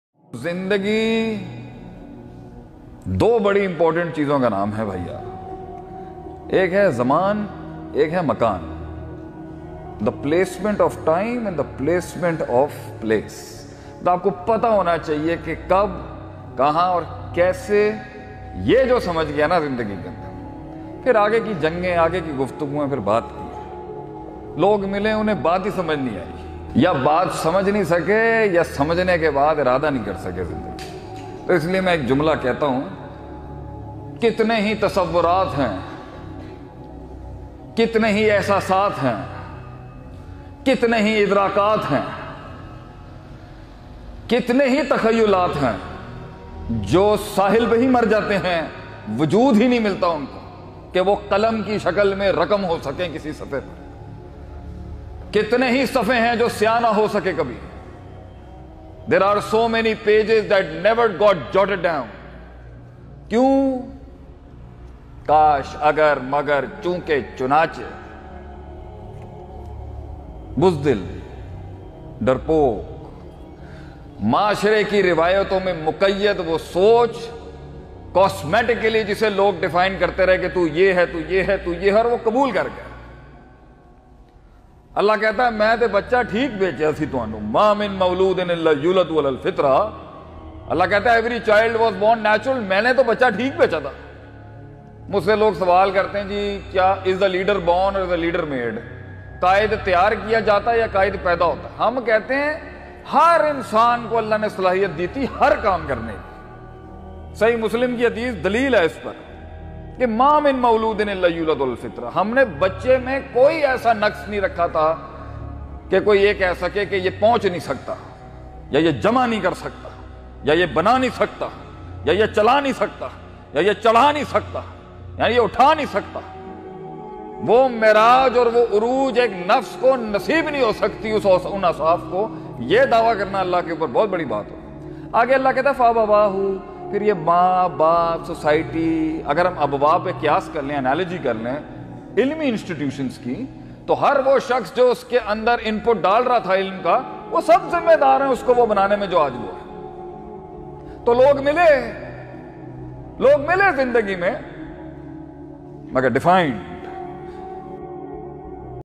Zindagi do bari important cheezun ka naam hai || Shaykh Atif Ahmad Butt || Motivation
Shaykh Atif explained the two important aspects of life.